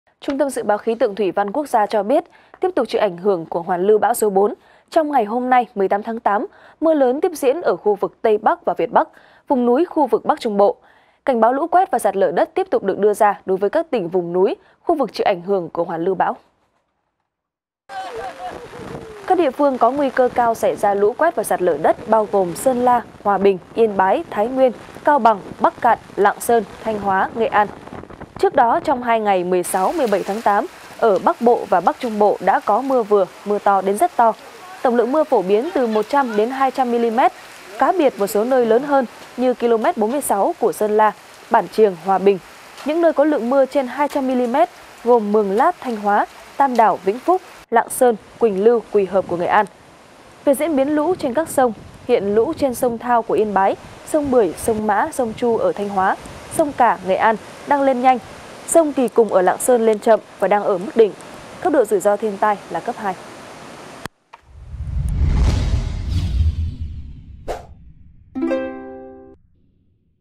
Mưa lớn tiếp tục tiếp diễn trong hôm nay, 18/8 | VTC14
VTC14 | Trung tâm Dự báo Khí tượng Thủy văn quốc gia cho biết, tiếp tục chịu ảnh hưởng của hoàn lưu bão số 4, trong ngày hôm nay 18/8, mưa lớn tiếp tục tiếp diễn ở khu vực Tây Bắc và Việt Bắc, vùng núi khu vực Bắc Trung Bộ. Cảnh báo lũ quét và sạt lở đất tiếp tục được đưa ra đối với các tỉnh vùng núi, khu vực chịu ảnh hưởng của hoàn lưu bão.